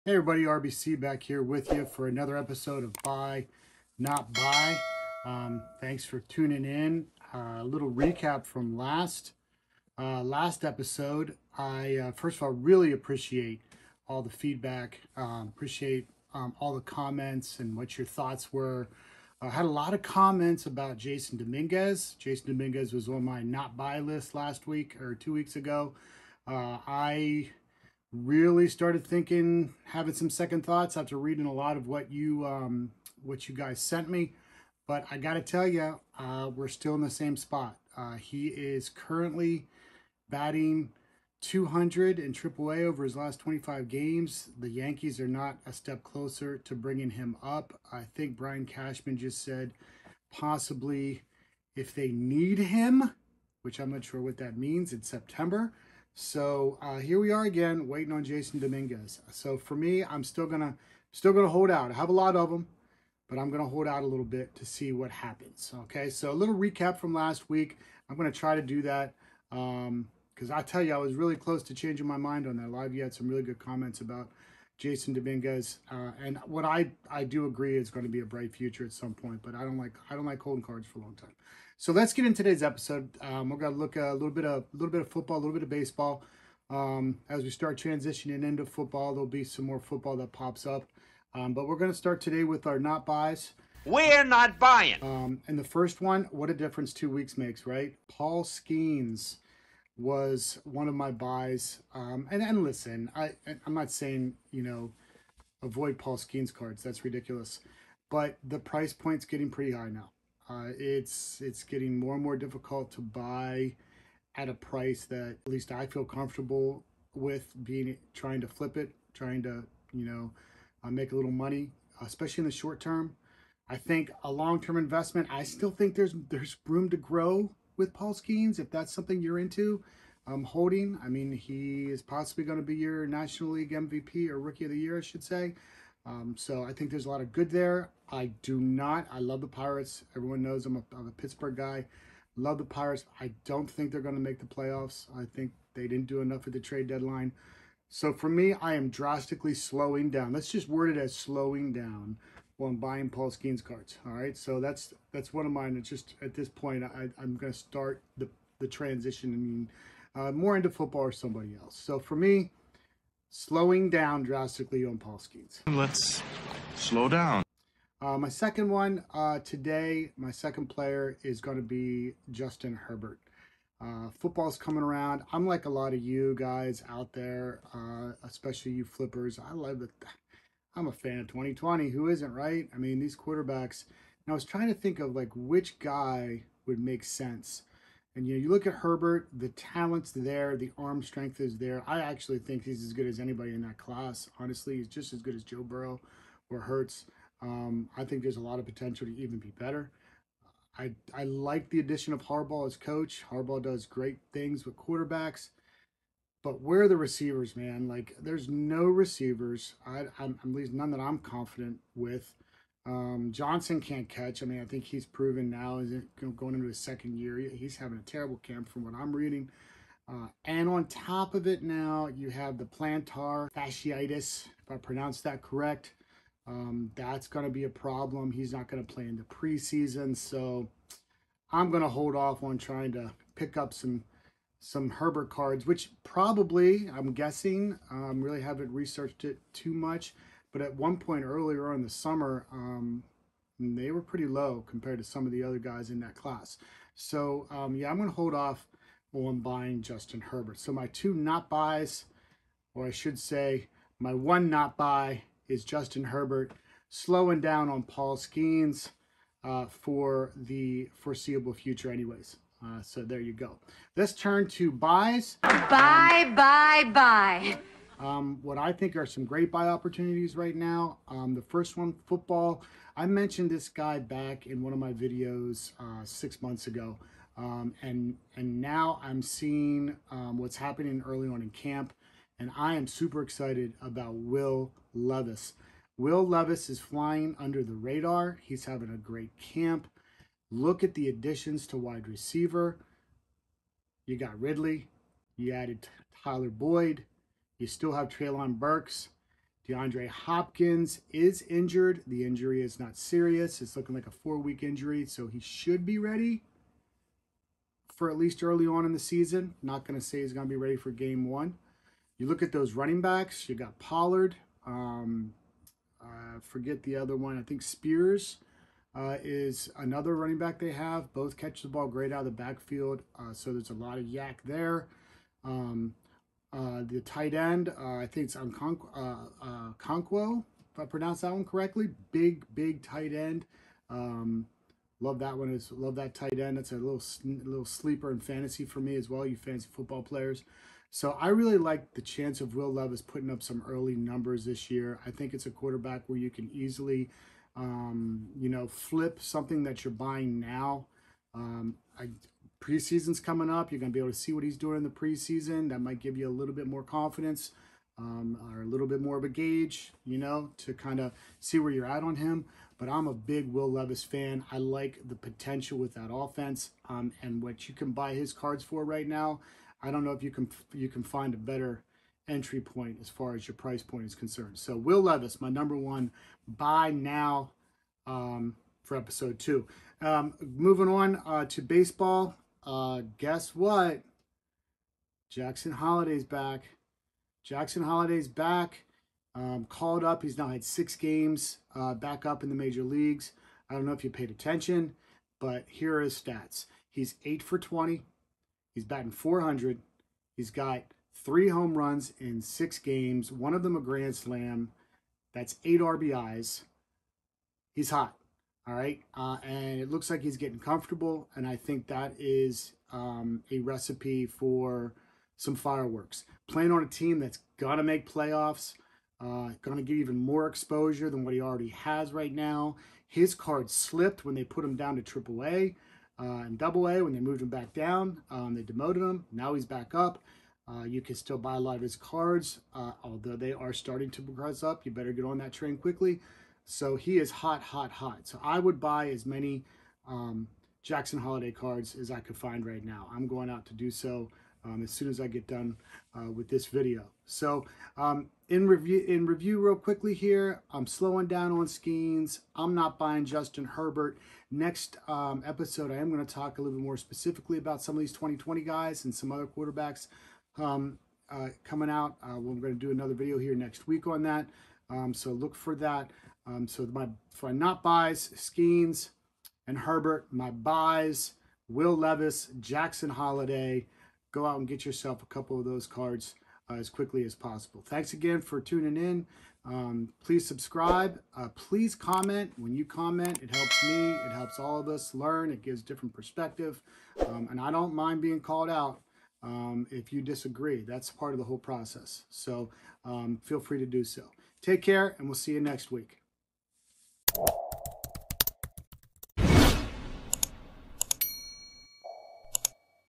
Cards I’m Buying and Not Buying! 🏀⚾️🏈 Episode 2
Episode 2

In this video, I share the sports cards I’m currently buying and those I’m avoiding. Learn about my investment strategies, market trends, and key insights into the sports card market. Whether you’re into rookie cards or looking for short-term or long-term investments, this video has something for every collector.

Top Loaders

Card Saver 1s

Ebay Store